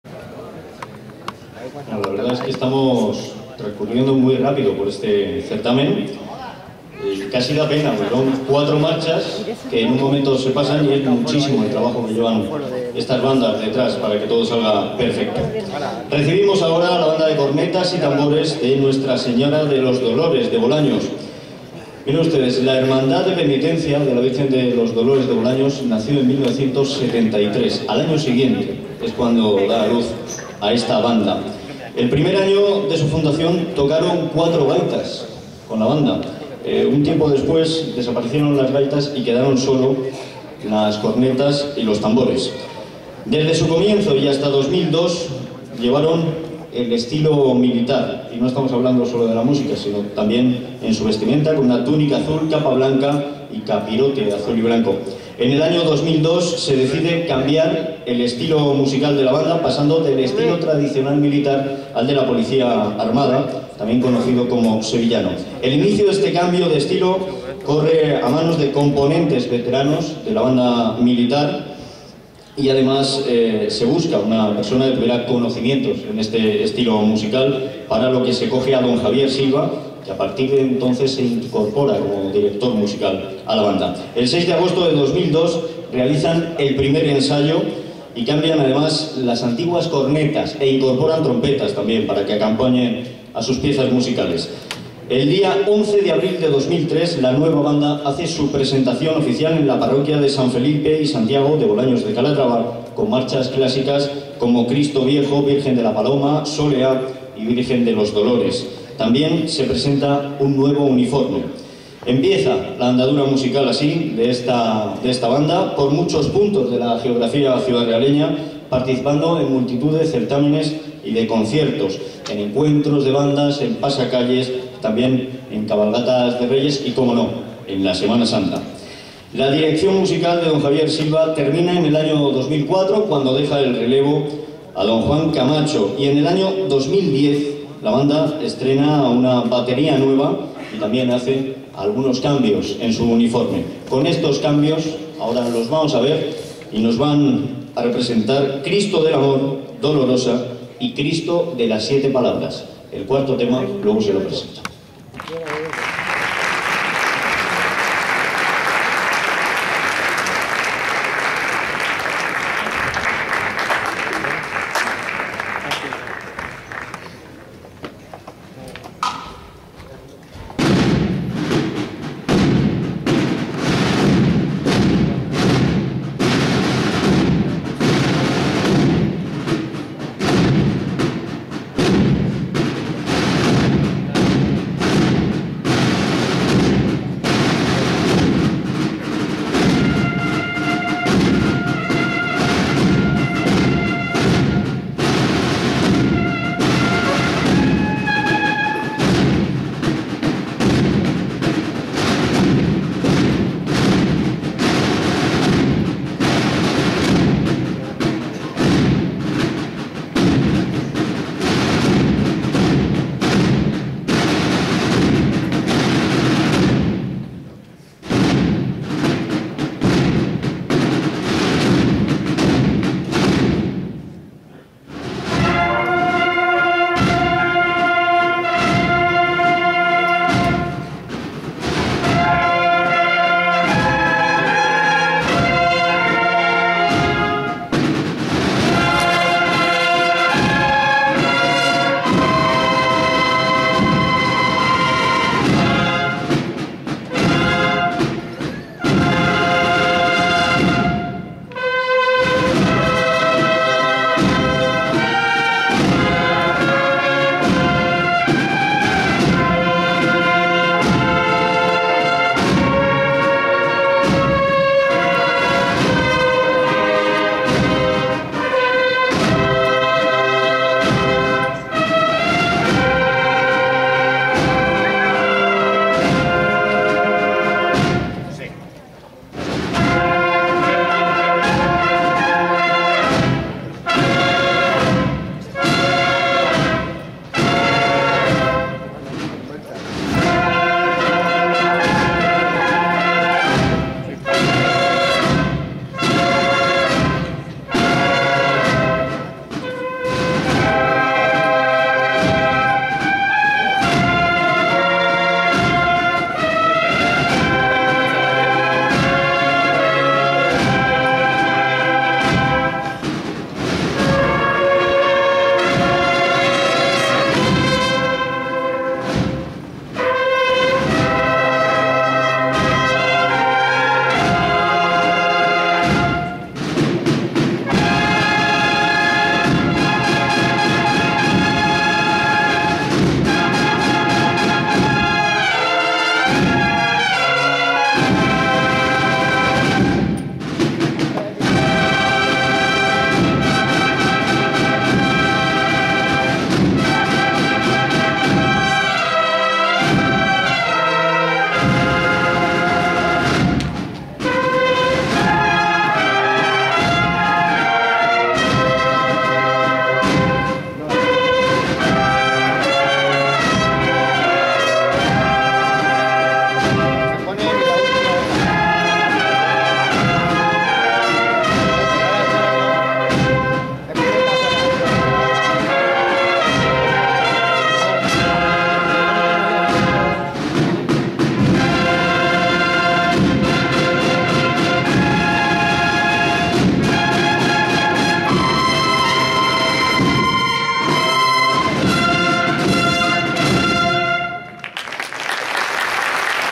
Banda CC y TT Nuestra Señora de los Dolores
VI Certamen Provincial de Bandas de CC y TT y Agrupaciones Musicales de Ciudad Real. Celebrado en Valdepeñas, suena la Banda CC y TT Nuestra Señora de los Dolores